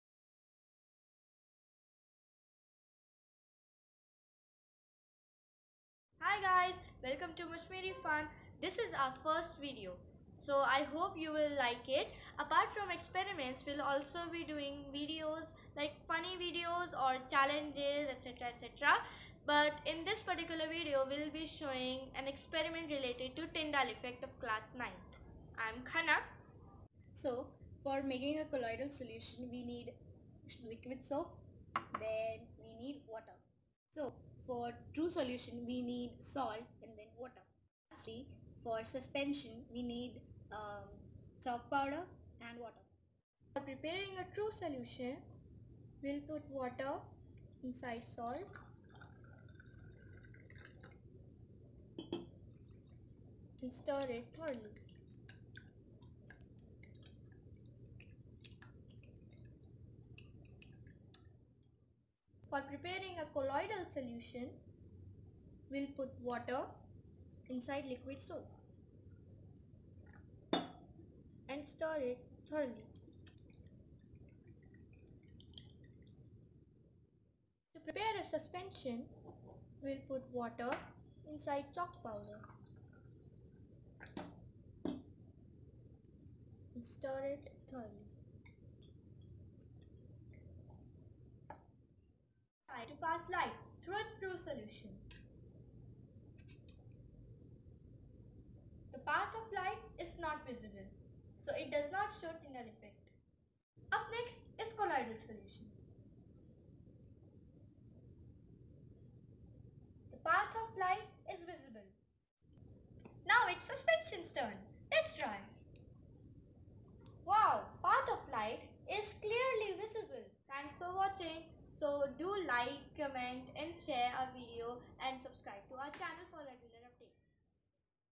TYNDALL EFFECT | Class 9 Activity | Mashmiri Fun
Hi Guys
This is our first video. Hope you like it and find it informative
This is based on 9th grade chemistry cbse
Tyndall effect is the process of scattering of light by minute particles present in a mixture
We will examine if a solution, colloid or suspension can scatter a beam of light